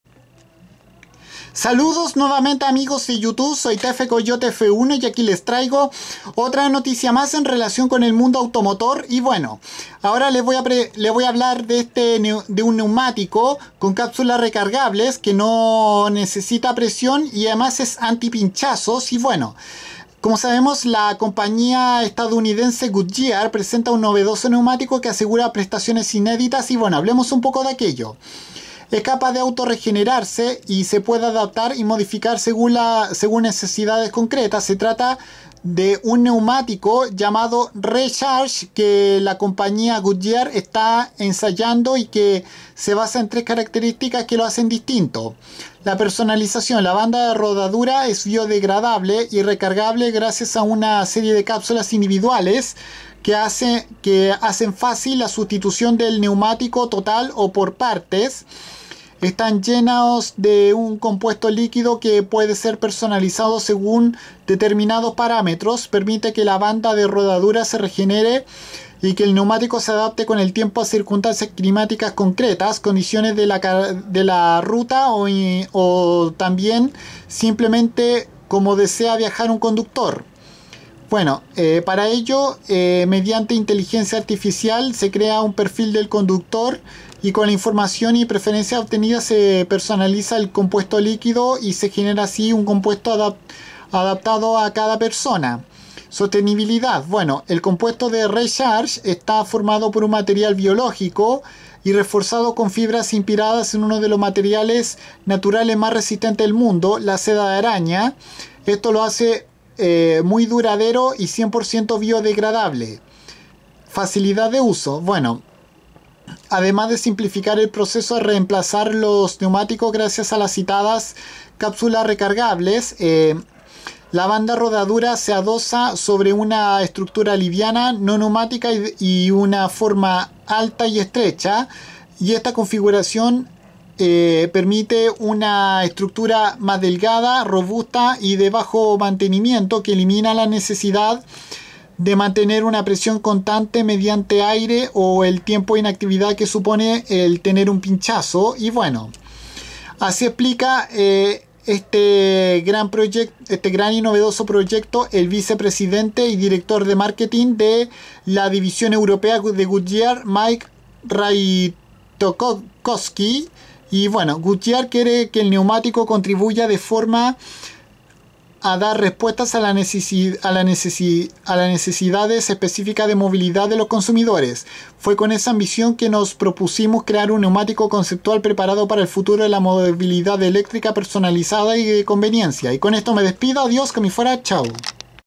Goodyear presenta un novedoso neumático con cápsulas recargables sin aire y antipinchazos
La emblemática marca estadounidense fabricante de neumáticos ha asegurado que este neumático genera inéditas prestaciones